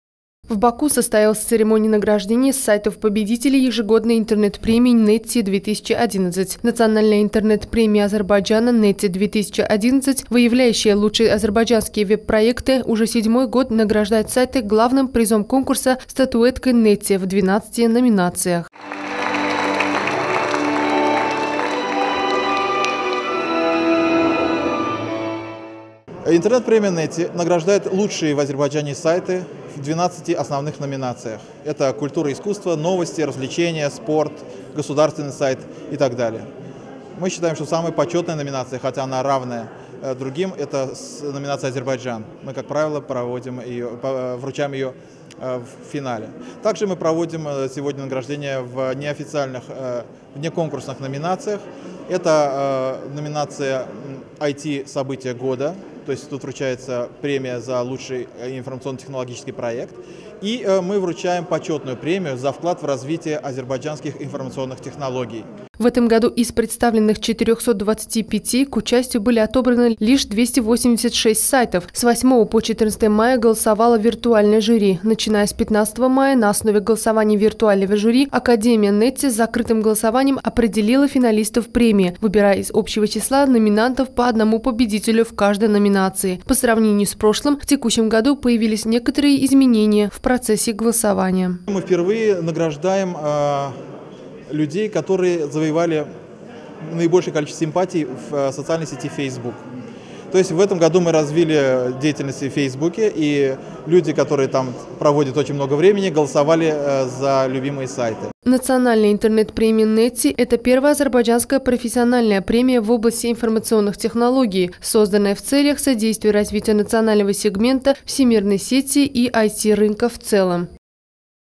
Netty-2011 at ATV International
Репортаж об Азербайджанской национальной интернет-премии Netty-2011, сделанный международным телеканалом ATV International.